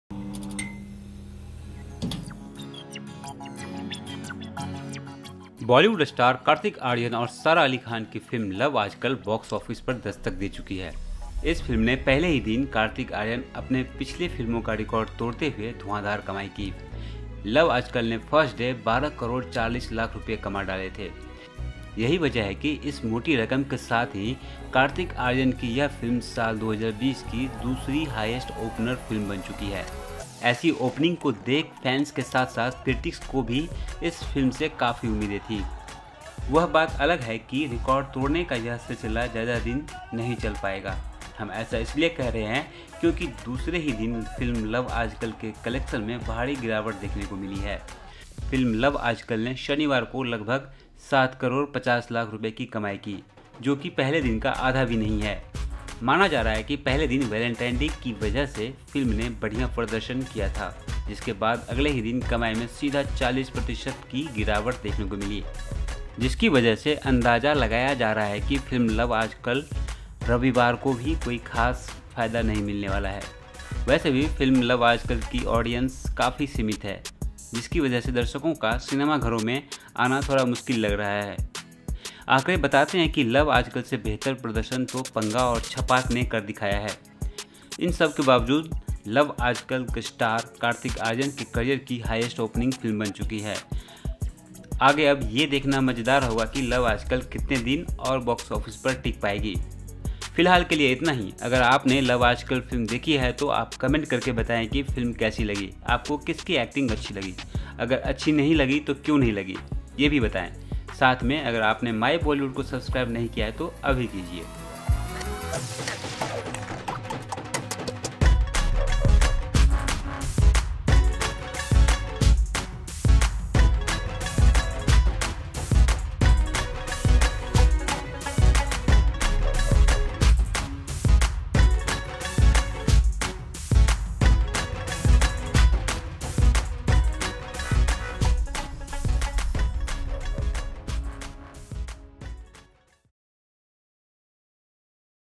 Love Aaj Kal 2 पहले दिन बंपर कमाई के बाद दूसरे दिन फुस्स
बॉलीवुड स्टार Kartik Aaryan और Sara Ali Khan की फिल्म  Love Aaj Kal ने पहले दिन जबरदस्त कमाई की लेकिन दूसरे दिन फुस्स हो गई .